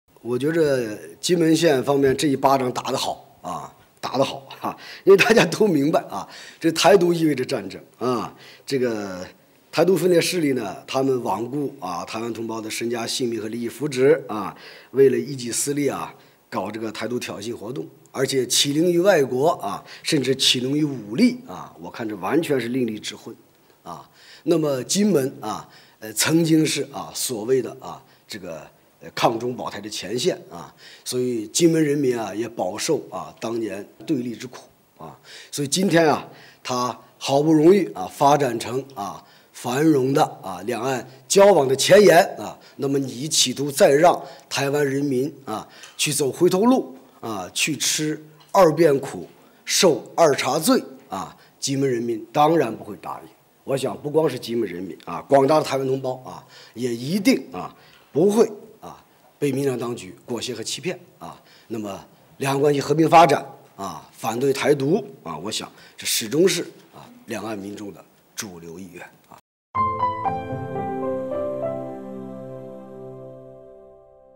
金门县长表示期盼和平不希望两岸有战事 国台办回应
在4月27日举行的国台办例行新闻发布会上，有记者提问：“日前，台陆军相关负责人以及有关民意代表组团考察金门防务，称金门马祖为‘两岸前线’须保‘台湾安全’。而金门县长则表示，金门人期盼和平，不希望两岸有战事，两岸应通过和平对话解决分歧。请问发言人对此有何评论？”
        国台办发言人马晓光对此回应称，金门县方面这一巴掌打得好。大家都明白，“台独”意味着战争，“台独”分裂势力罔顾台湾同胞的身家性命和利益福祉，为一己私利搞“台独”挑衅活动，而且乞灵于外部势力甚至于武力，完全是利令智昏。
        马晓光说，金门曾经是所谓的“抗中保台”前线，金门人民饱受当年对立对抗之苦，今天好不容易繁荣发展成为两岸交往的前沿。企图让他们走回头路，吃二遍苦，受二茬罪，金门人民当然不会答应。不光是金门人民，广大的台湾同胞也一定不会被民进党当局裹挟和欺骗，两岸关系和平发展、反对“台独”，这始终是两岸民众的主流意愿。